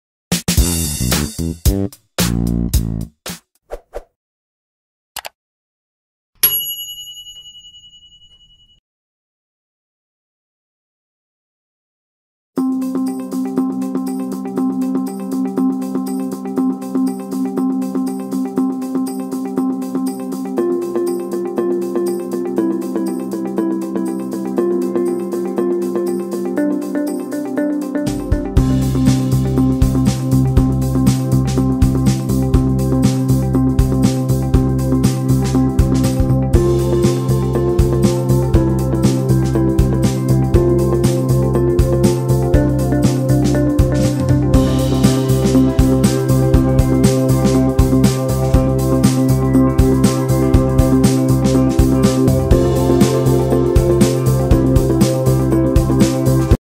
Iklan Real Estate, Perumahan, Pemasaran Rumah Cinematic, Motion Graphic
Iklan Real Estate, Perumahan, Pemasaran Rumah Cinematic, Motion Graphic. Video berikut adalah video company profile perusahaan digital. Nama perusahaan di video ini hanyalah fiktif belaka. Hanya sebagai contoh saja.